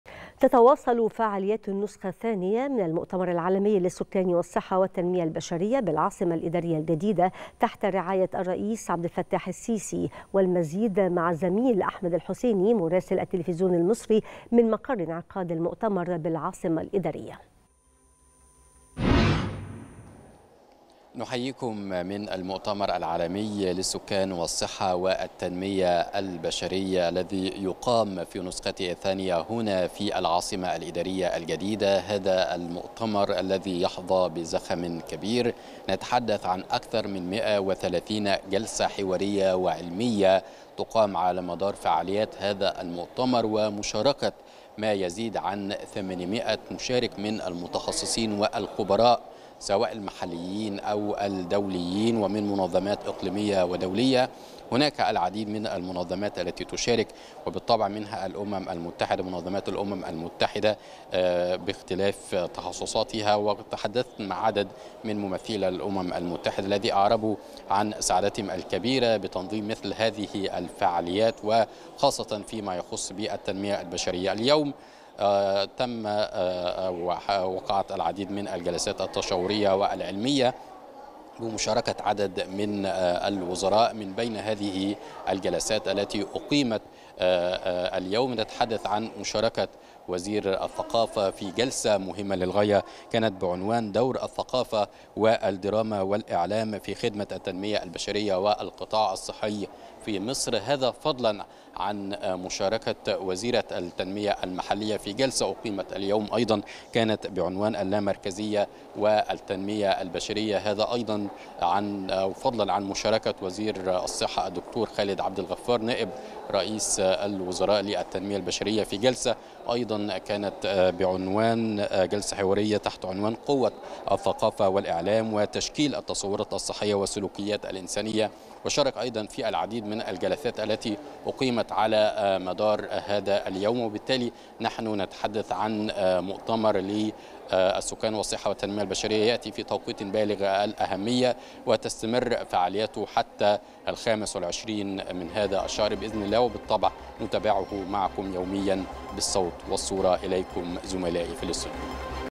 تواصل فعاليات المؤتمر العالمي للسكان والصحة والتنمية البشرية في نسخته الثانية
تواصل فعاليات المؤتمر العالمي للسكان والصحة والتنمية البشرية في نسخته الثانية.. مراسل التليفزيون المصري يوضح التفاصيل